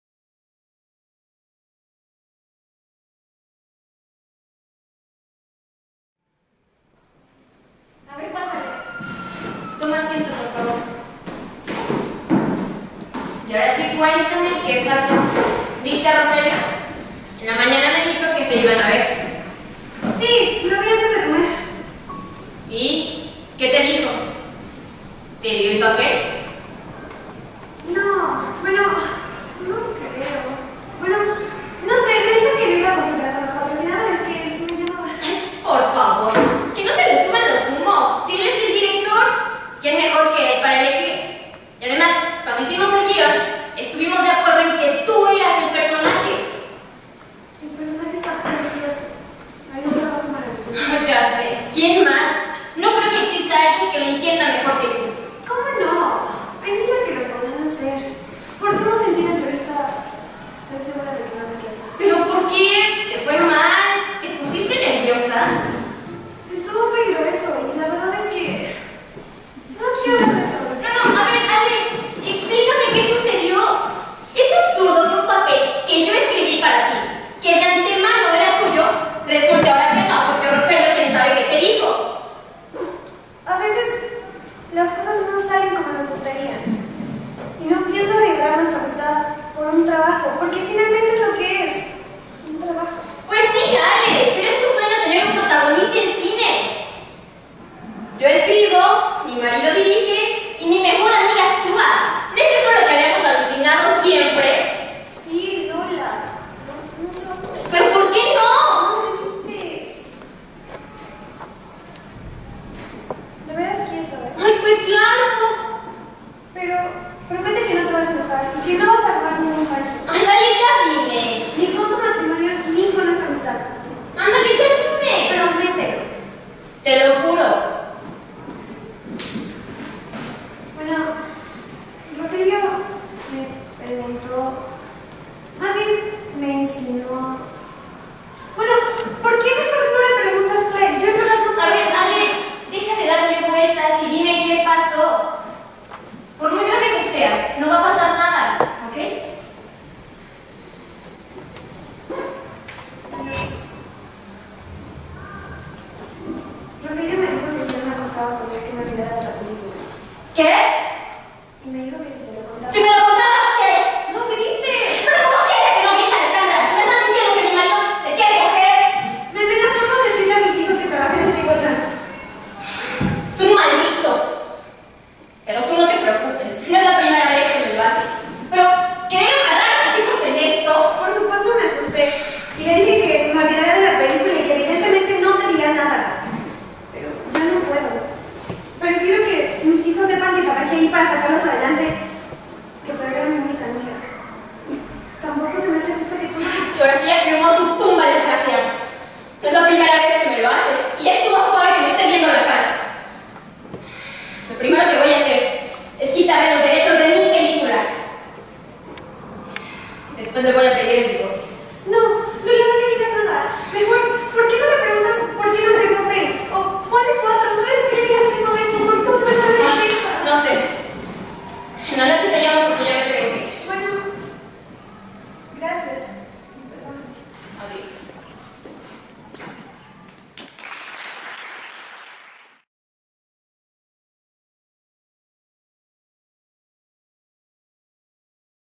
Escena corta Ale, Lola y el papel. Grupo teatral Escena XXI prepa 4
Escena teatral presentada en febrero de 2014. De alumnas de 6to grado, para alumnos de 4to grado. En ENP plantel 4.